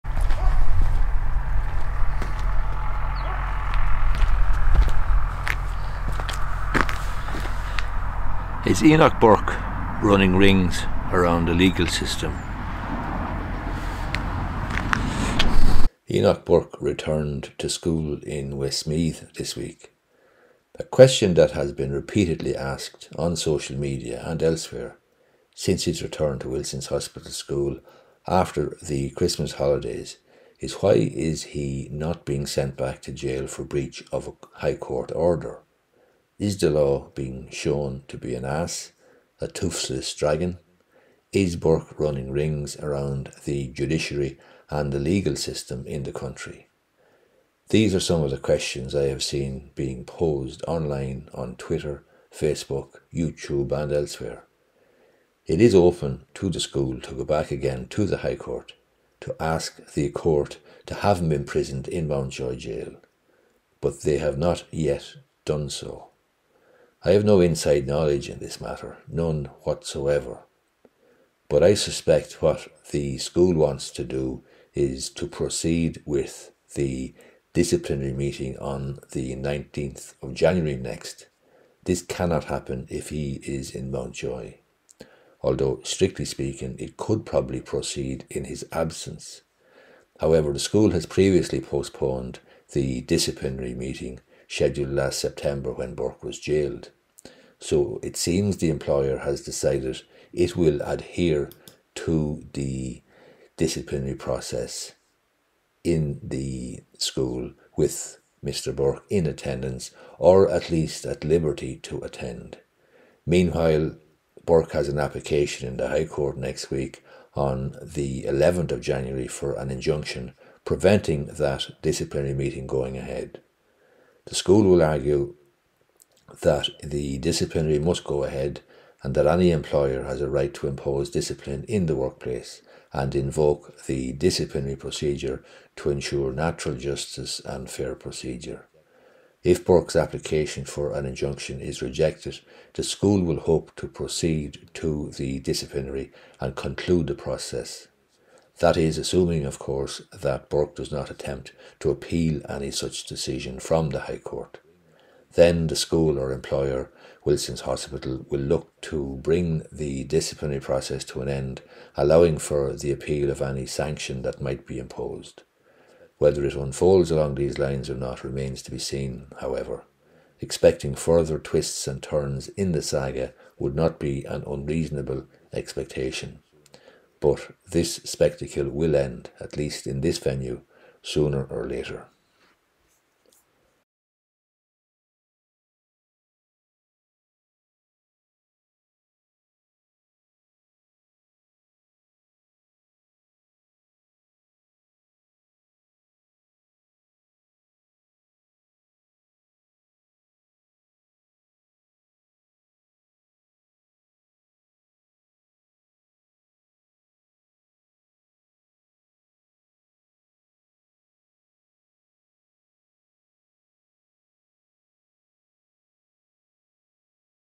Is Enoch Burke running rings around the law and the legal system in Ireland?
27 Irish Employment Law Cases: Priceless Lessons for Employers and Employees from Decided Cases of the EAT, Equality Tribunal, and High Court